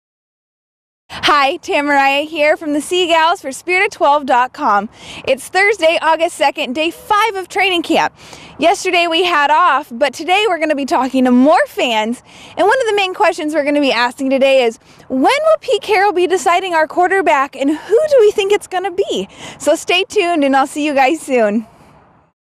2012 Seahawks Training Camp - Day 5
Checking in with what's ahead on Day 5 of camp!